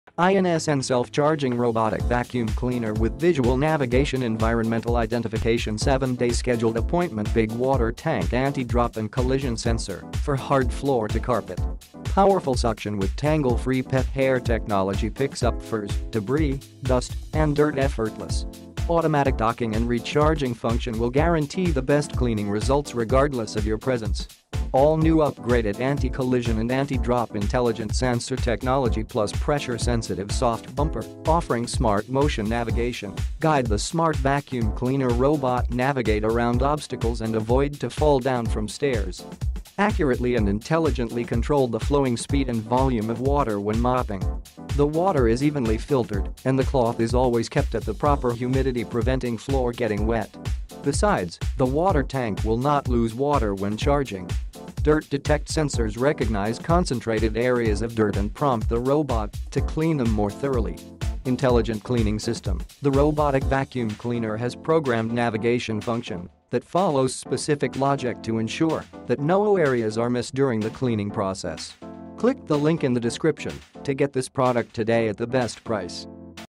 INSN Self Charging Robotic Vacuum Cleaner with Visual Navigation,Environmental Identification,7 Day
INSN Self-Charging Robotic Vacuum Cleaner with Visual Navigation,Environmental Identification,7-Day Scheduled Appointment,Big Water Tank,Anti-Drop & Collision Sensor, for Hard Floor to Carpet ★Powerful suction with tangle-free pet hair technology picks up furs, debris, dust and dirt effortless. Automatic docking and recharging function will guarantee the best cleaning results regardless of your presence.
★All-new upgraded anti-collision & anti-drop intelligent sensor technology plus pressure-sensitive soft bumper, offering smart motion navigation, guide the smart vacuum cleaner robot navigate around obstacles and avoid to fall down from stairs.
★Accurately and intelligently control the flowing speed and volume of water when mopping. The water is evenly filtered and the cloth is always kept at the proper humidity preventing floor getting wet. Besides, the water tank will not lose water when charging.
★Dirt detect sensors recognize concentrated areas of dirt and prompt the robot to clean them more thoroughly.
★Intelligent Cleaning System: The robotic vacuum cleaner has programmed navigation function that follows specific logic to ensure that no areas are missed during the cleaning process."